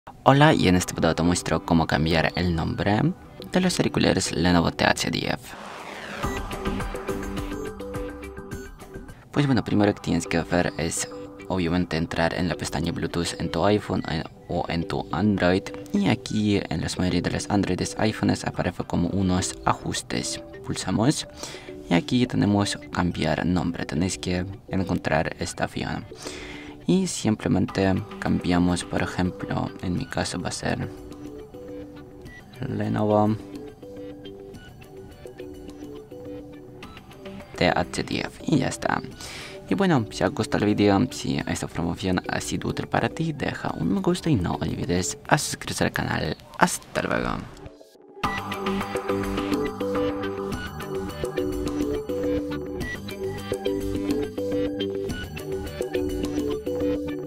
Cómo cambiar el nombre del dispositivo en LENOVO TH10
¡Aprende a editar el nombre del dispositivo en tu LENOVO TH10! En este video, te mostraremos cómo personalizar el nombre de tu dispositivo para que se ajuste a tus preferencias.

¿Dónde encuentro la opción para editar el nombre de mi LENOVO TH10?
¿Puedo personalizar el nombre de mi dispositivo LENOVO TH10?
¿Cómo modificar el nombre del teléfono en LENOVO TH10?